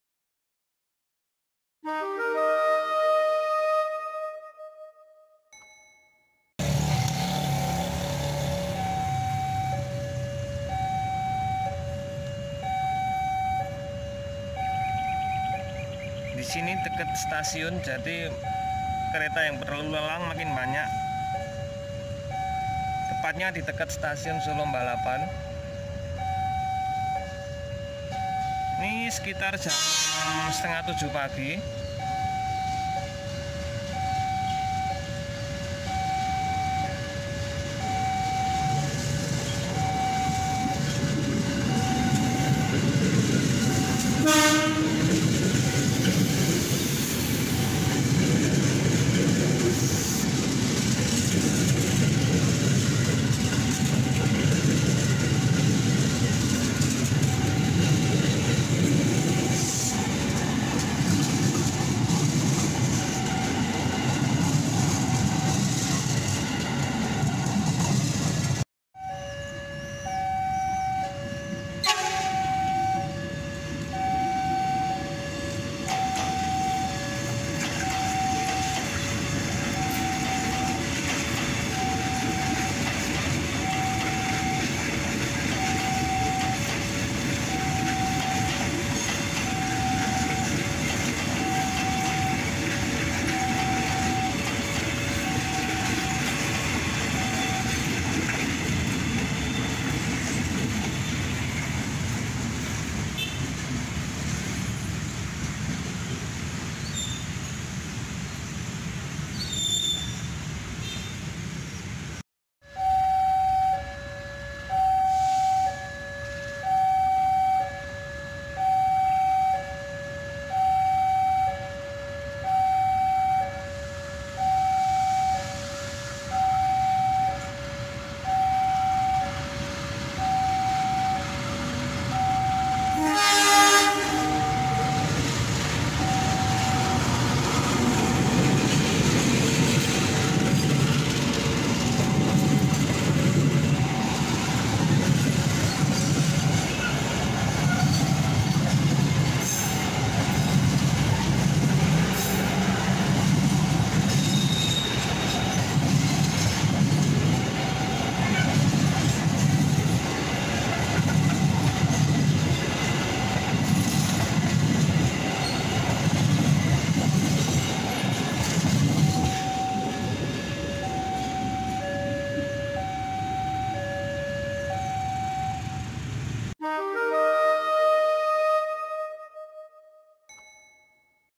kereta api krl panjang keren jogja solo jogja indonesia
KRL jogja solo ini dalam sehari memiliki 20 perjalanan dan 11 stasiun pemberhentian yaitu :
Stasiun Yogyakarta
Stasiun Lempuyangan
Stasiun Maguwo
Stasiun Brambanan
Stasiun Srowot
Stasiun Klaten
Stasiun Ceper
Stasiun Delanggu
Stasiun Gawok
Stasiun Purwosari
Stasiun Solo Balapan.

diakhir video ada bonus kereta api joglosemarkerto, keren...

Keterangan Produksi
Tanggal : Edisi 2022
Lokasi   :  Sisi Barat Stasiun Solo-Balapan
Pengambilan Video Sudah Mengutamakan Aspek Keselamatan

Terimakasih